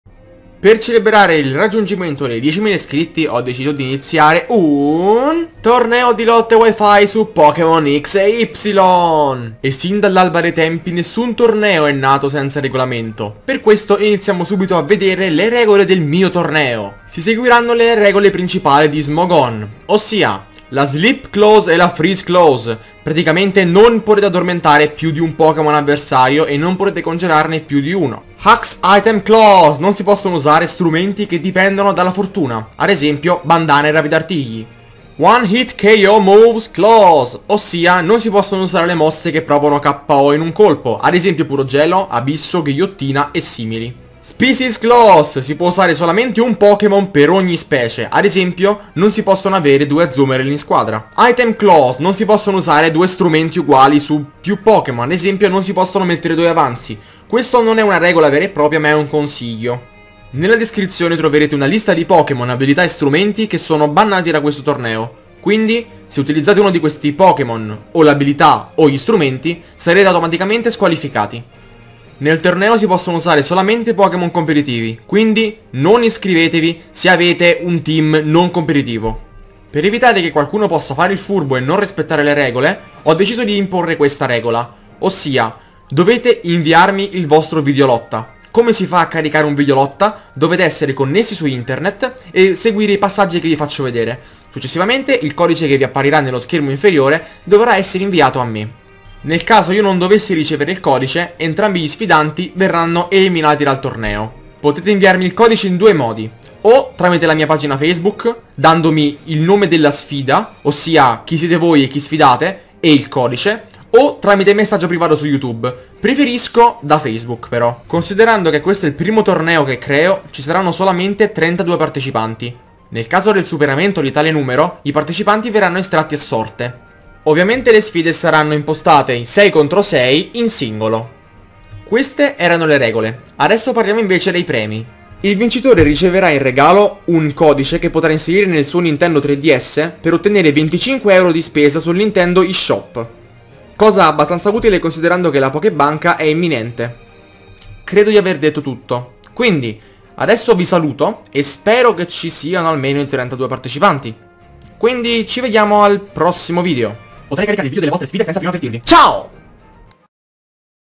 Pokemon X & Y Wi-Fi Tournament [ISCRIZIONI CHIUSE]
LEGGERE LA DESCRIZIONE!

Ovviamente per iscrivervi dovete commentare qua sotto.

Potete iscrivervi entro il 18 Gennaio 2014. Il 19 rivelo i partecipanti. Se avete qualche domanda, chiedete pure nei commenti. Ovviamente io non parteciperò.

Regole e altro nel video.

BANLIST:

Pokemon

-Arceus
-Blaziken
-Darkrai
-Deoxys
-Deoxys-A
-Dialga
-Giratina
-Giratina-O
-Groudon
-Ho-oh
-Kyogre
-Kyurem-W
-Lugia
-Mewtwo
-Palkia
-Rayquaza
-Reshiram
-Shaymin-S
-Xerneas
-Yveltal
-Zekrom

Abilità

-Altalena

Strumenti

-Gengarite
-Kangaskhanite